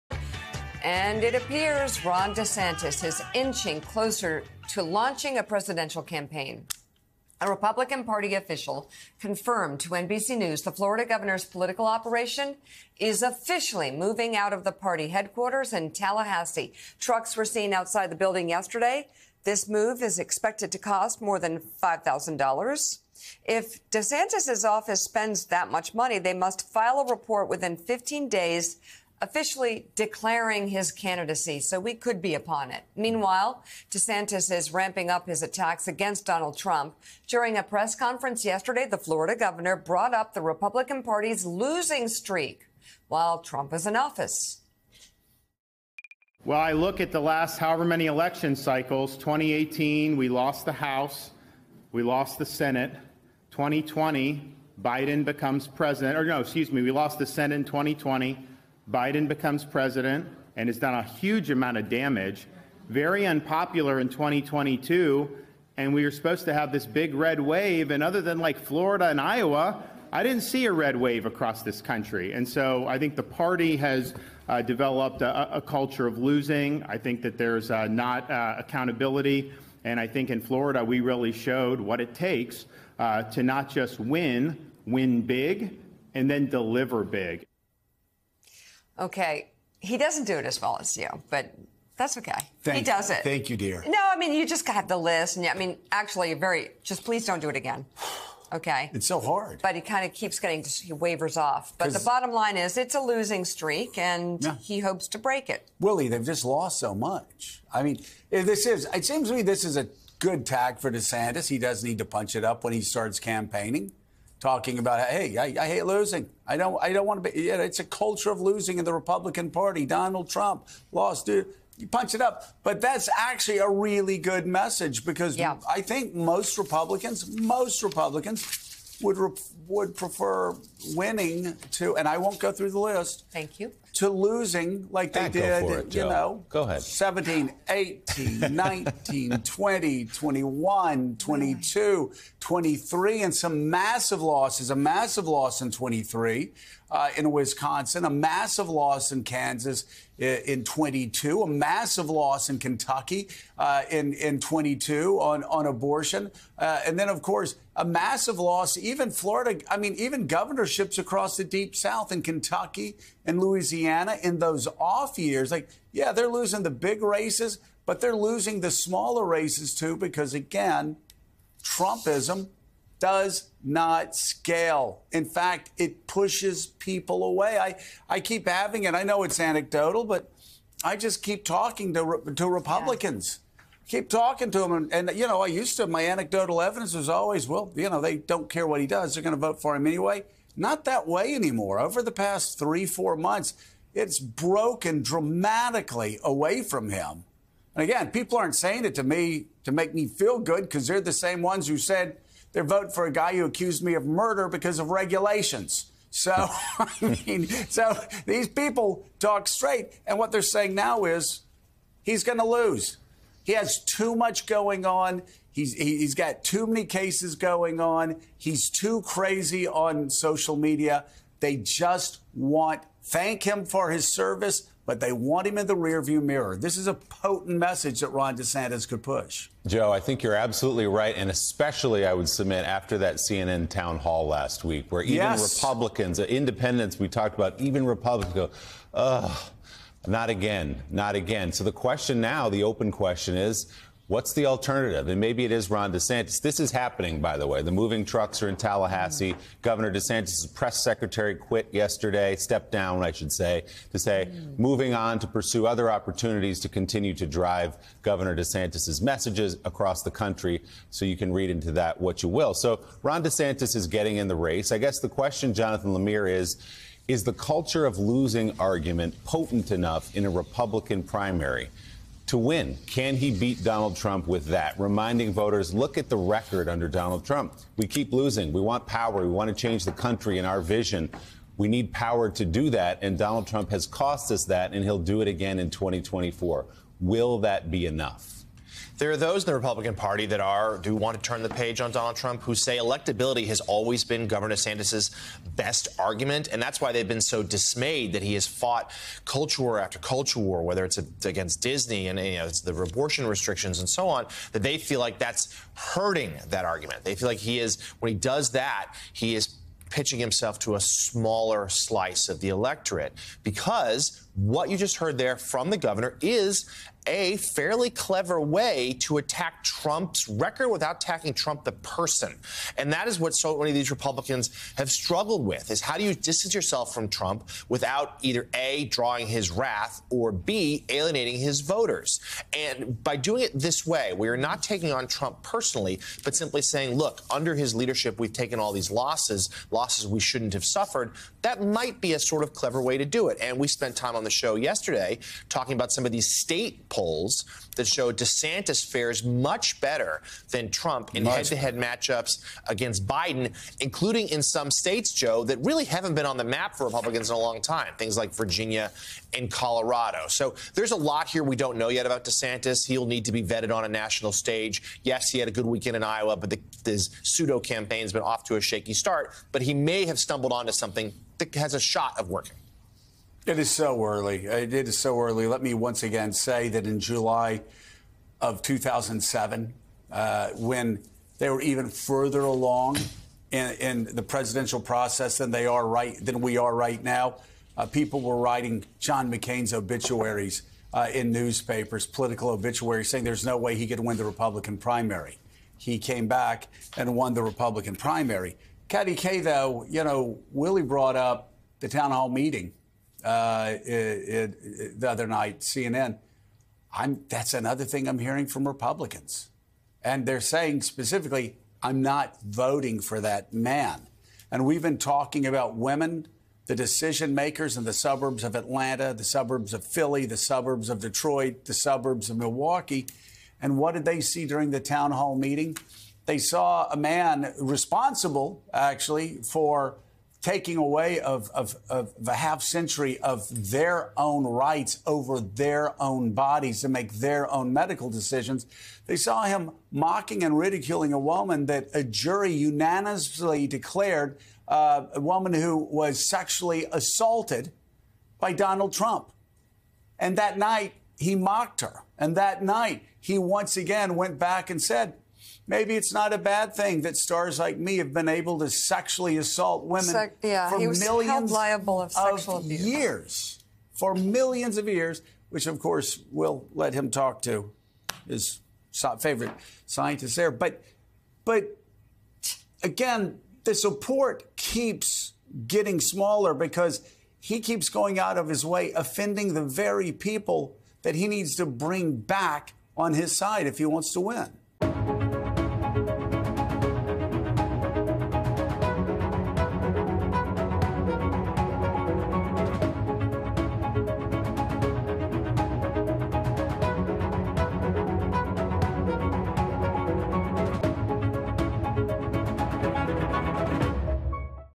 DeSantis could have a potent message talking about GOP's 'culture of losing'
Gov. Ron DeSantis, R-Fla., has yet to formally announce his bid for 2024, but he could possibly have a strong message on his hands in talking about changing the Republican Party's culture of losing.